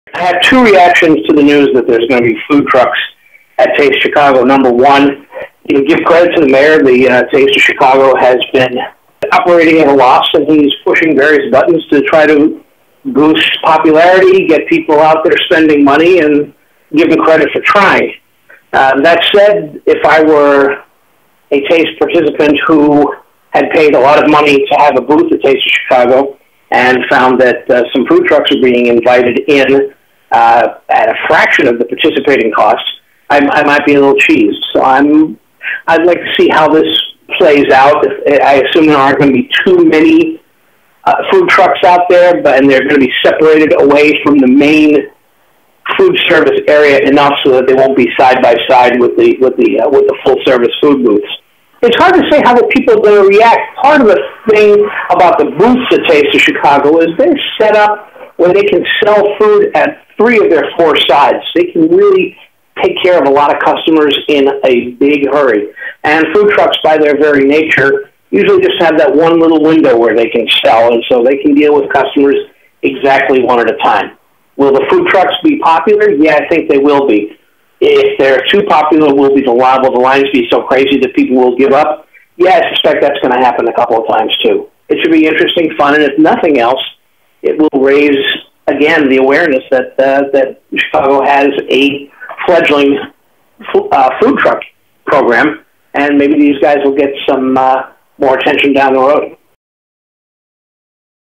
Food trucks at Taste of Chicago could be a hit
Chicago Tribune food critic Phil Vettel reacts to food trucks being added to the Taste of Chicago. (Posted: April 12, 2013)